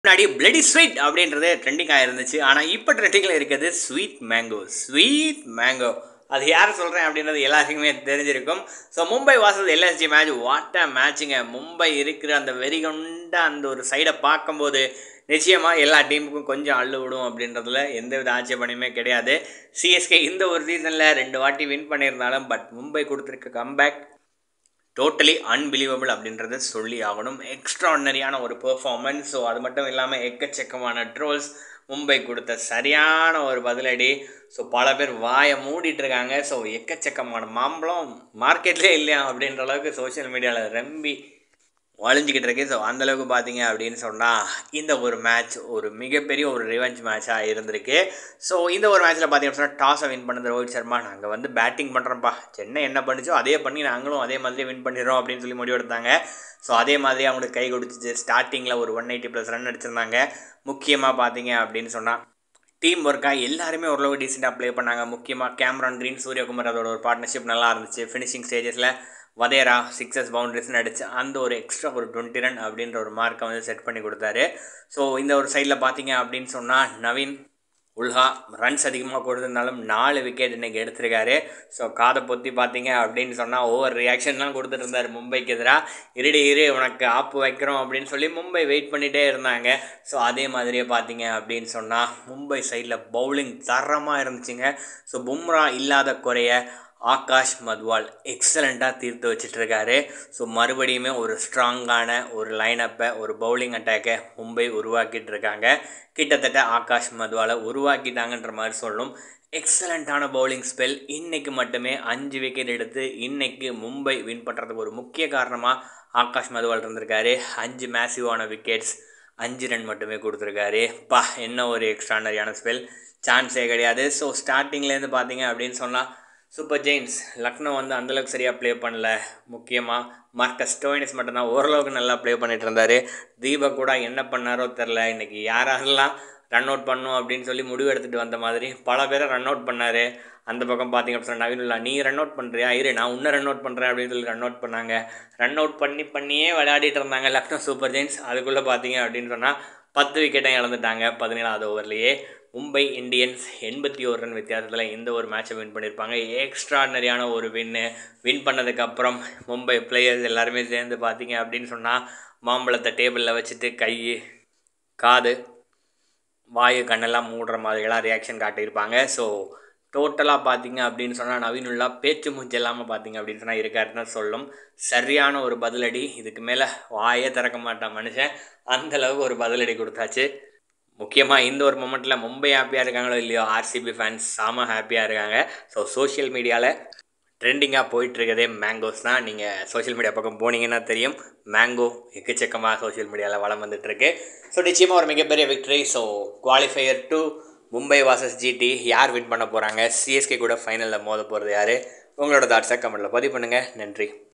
சரியான செருப்படி கொடுத்த மும்பை! கொண்டாட்டத்தில் கோலி, RCB Fans | MI vs LSG Highlights, IPL 2023
mi vs lsg match highlights tamil ipl 2023 eliminator best revenge mangoes sold out gambir